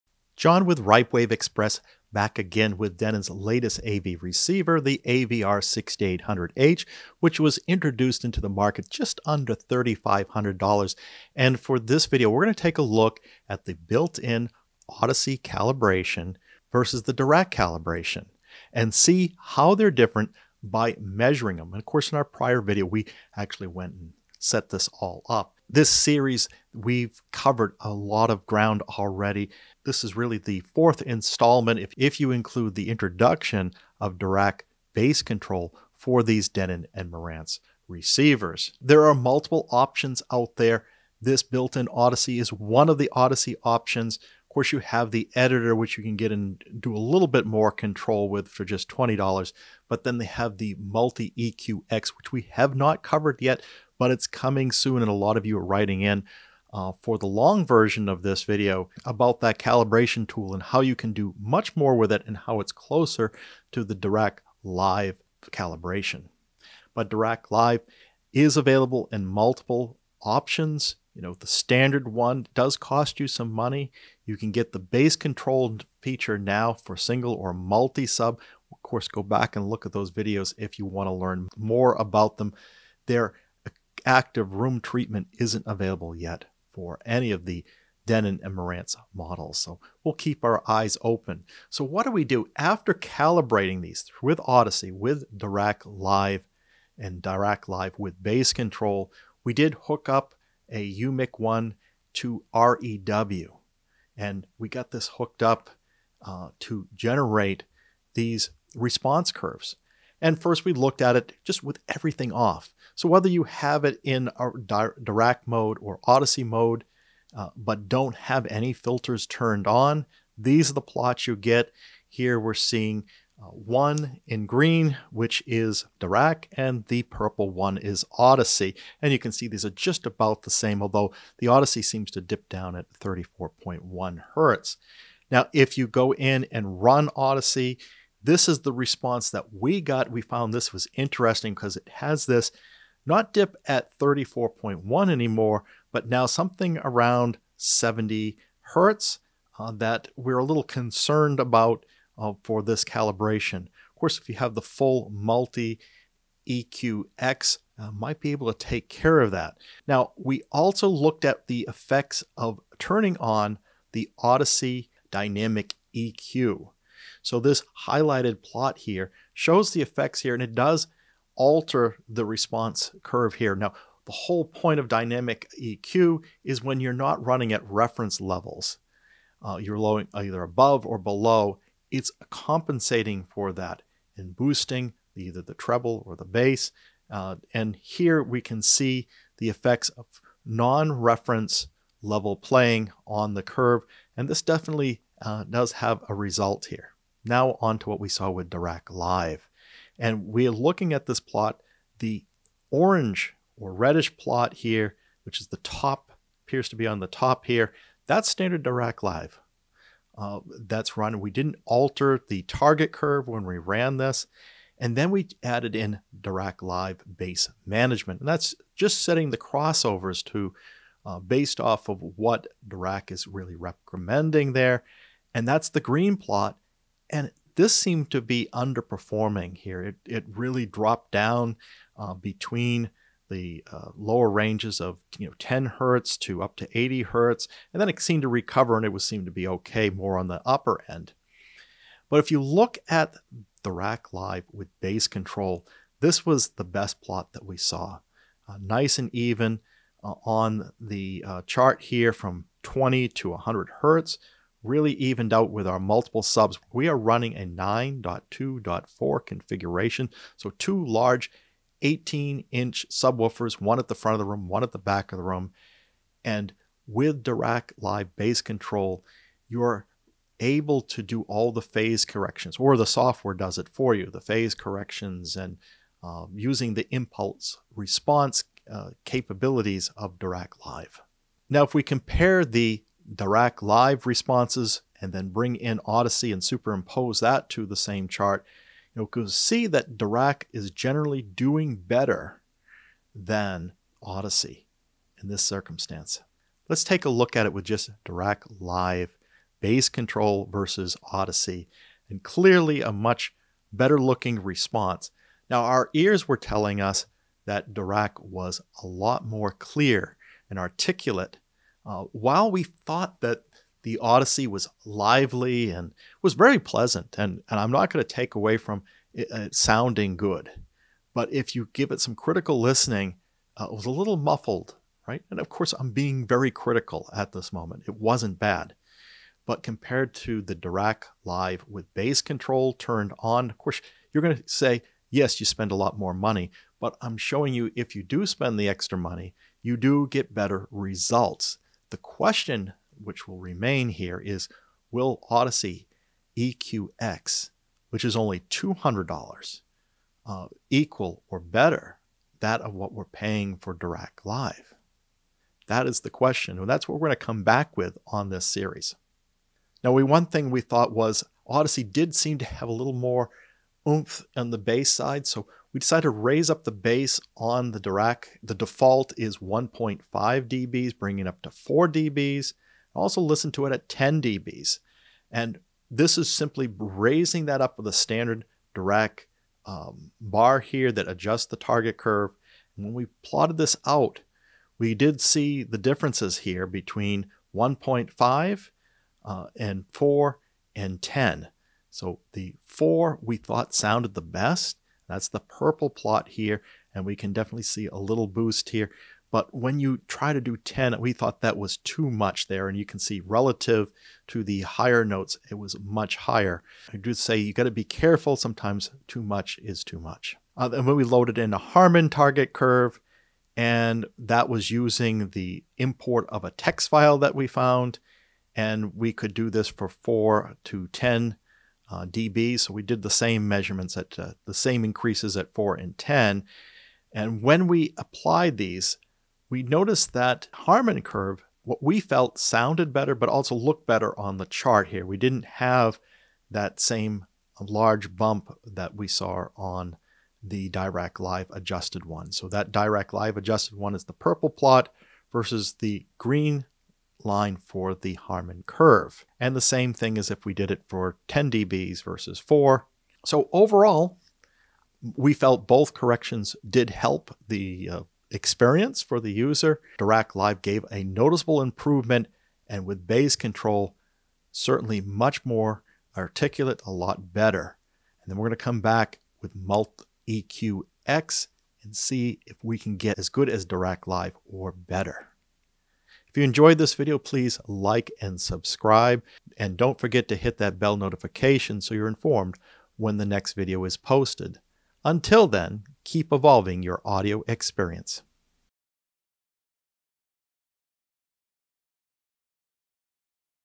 Denon AVR-X6800H Xpress Review Audyssey and Dirac Calibrations Compared
In this video we compare Audyssey, Dirac Live, Dirac Live Bass Managed and Dirac Live Bass Control calibrations using REW to measure the actual results.